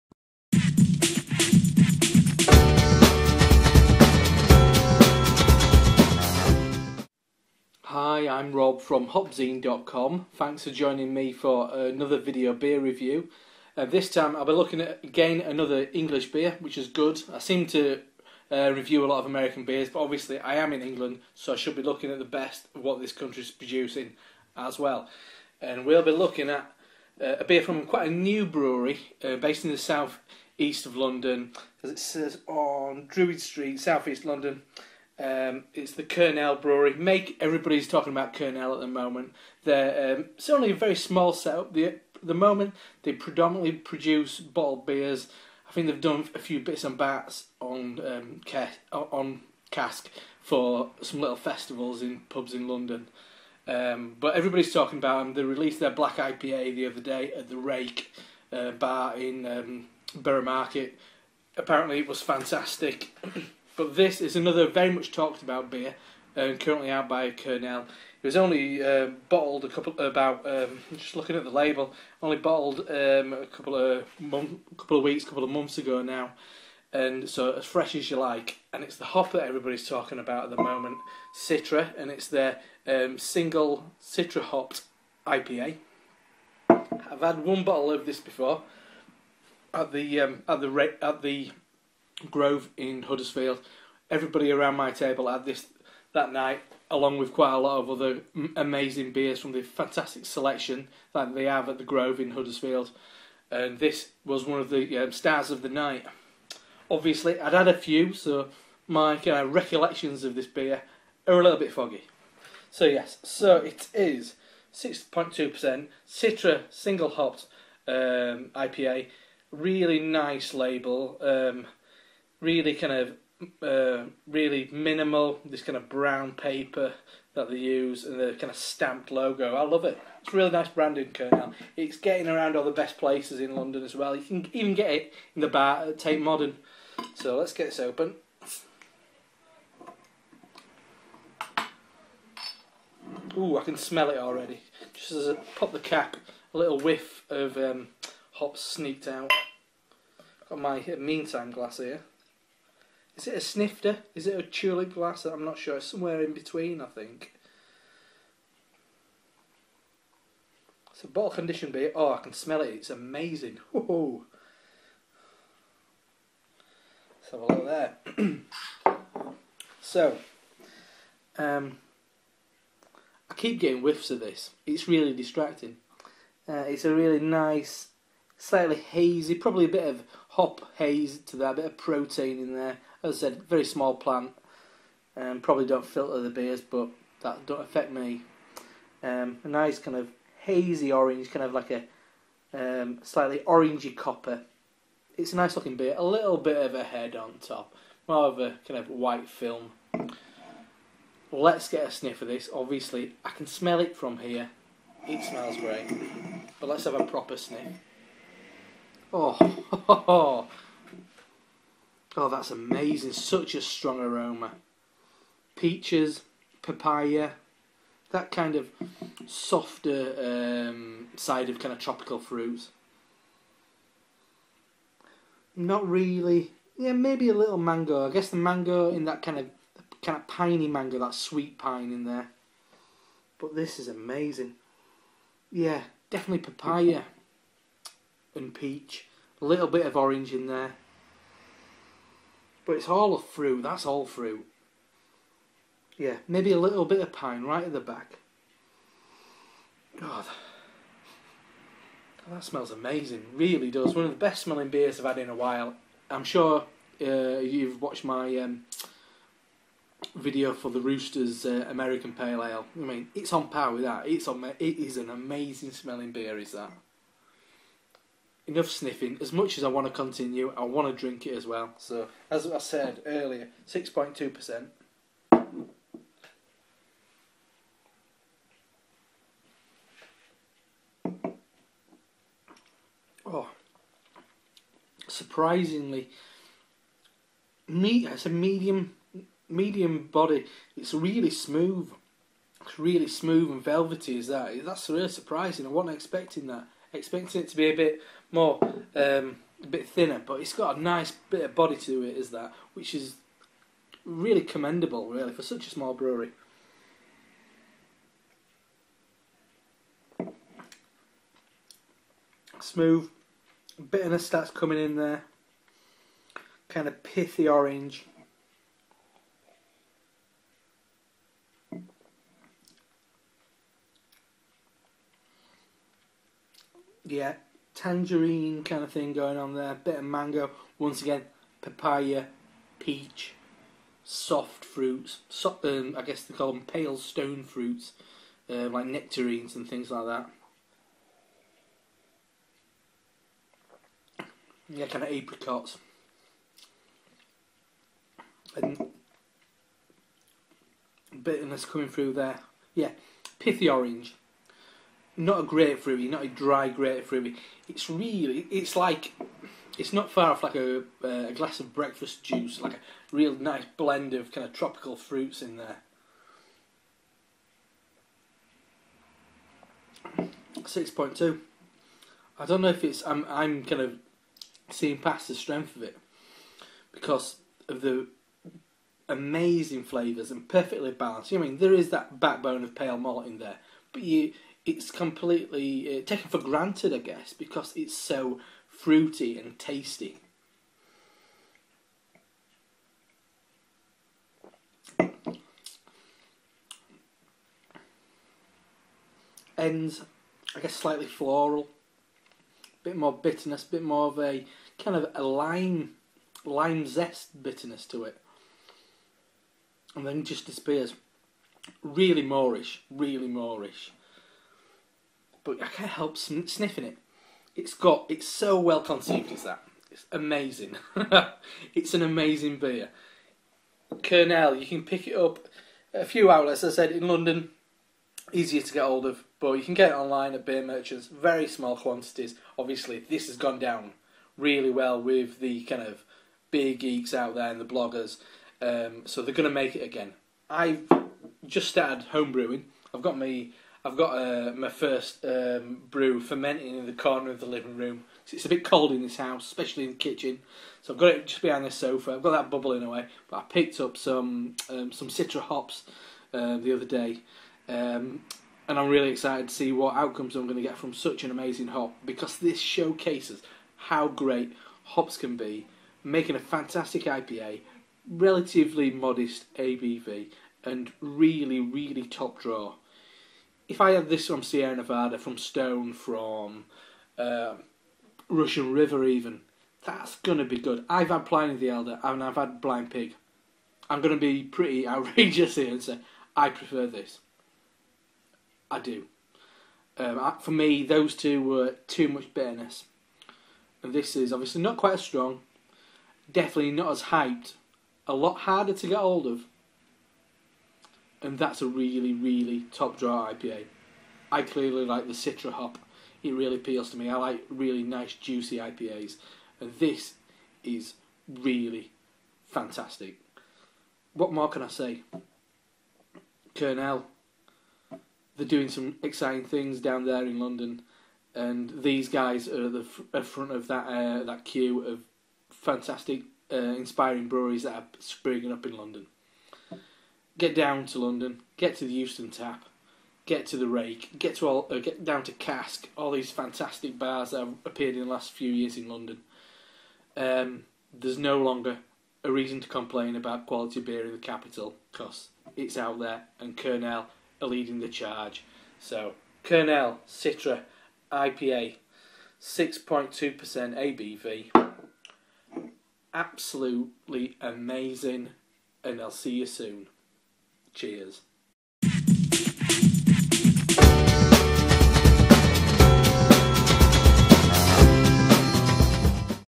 Kernel Citra IPA Review
Thanks for joining me for another Video Beer Review. This I'll be drinking a bottle of The Kernel Brewery's Citra single hop IPA.